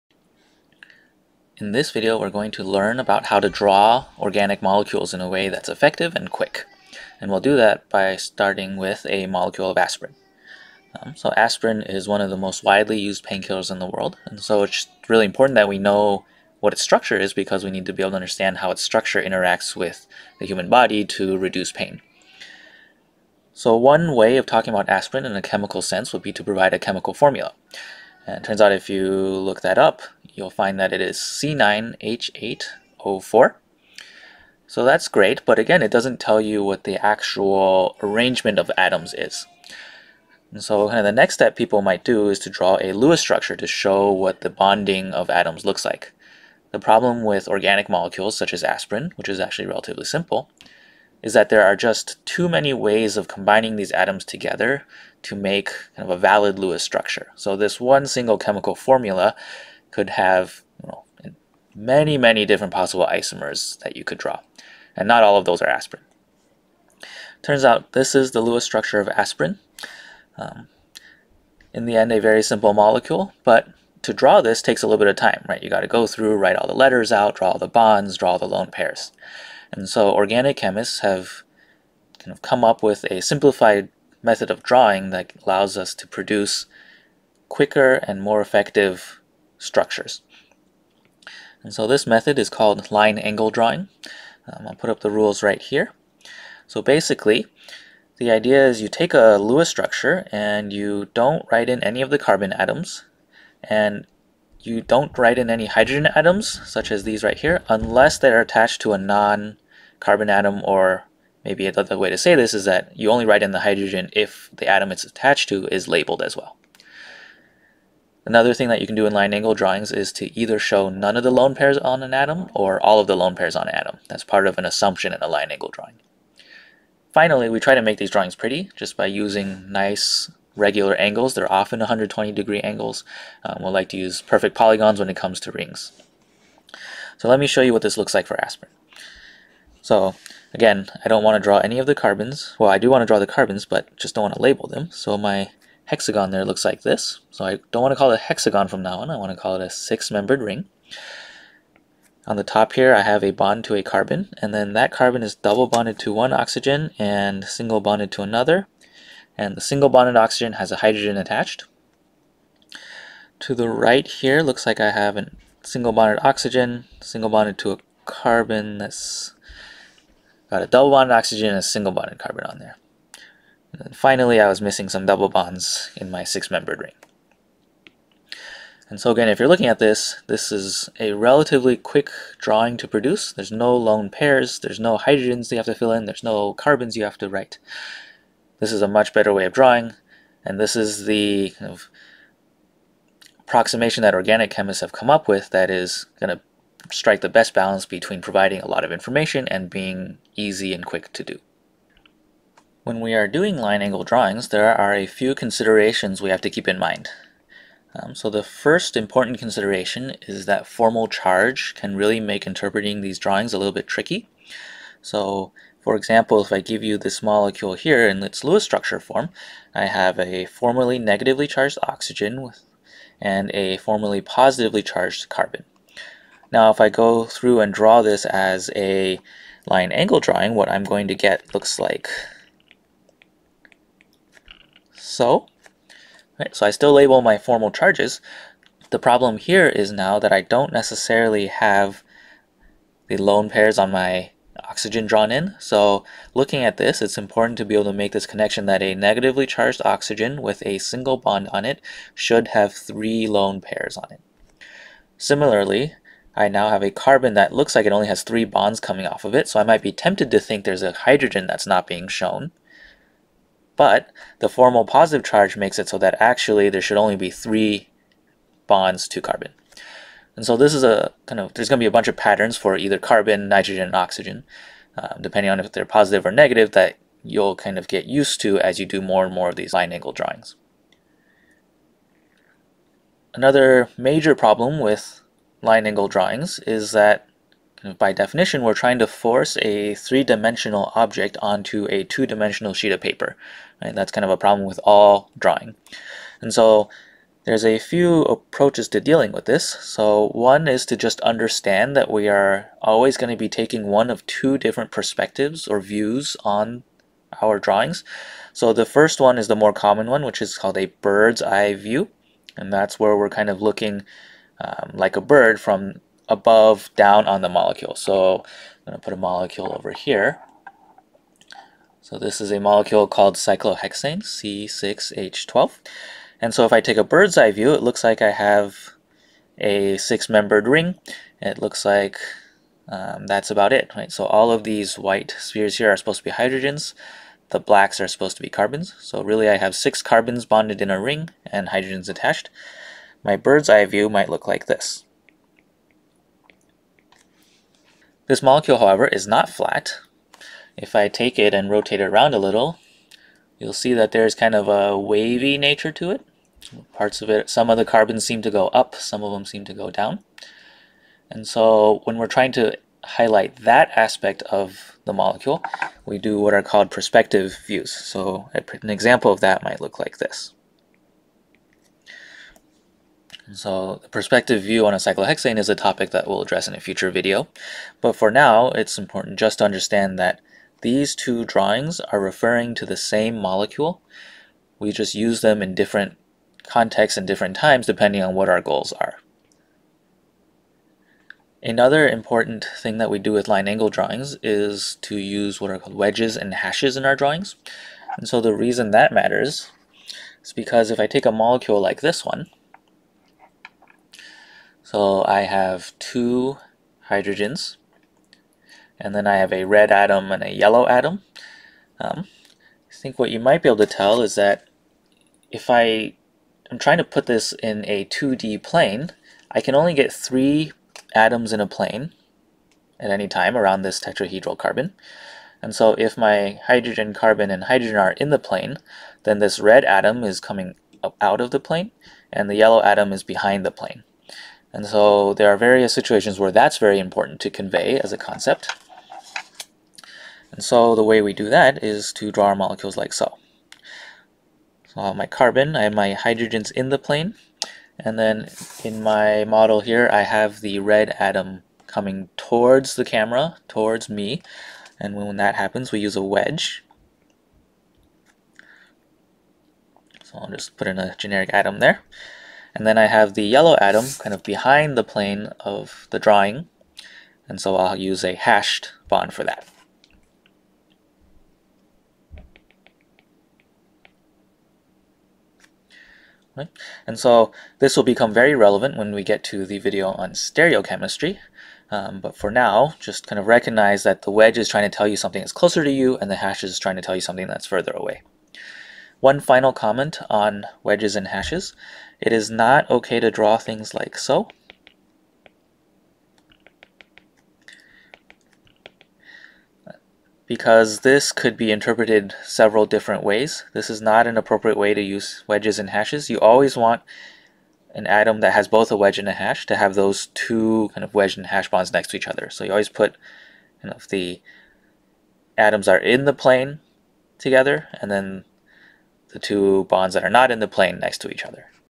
Chem 211 - Day 1 - Drawing - Organic Chemistry 1
This video goes over how to do line angle drawings of organic molecules.

You will have the most success learning from this video if you pause frequently to take notes and digest the material.

I made these videos for pandemic teaching of Introductory Organic Chemistry at Macalester College.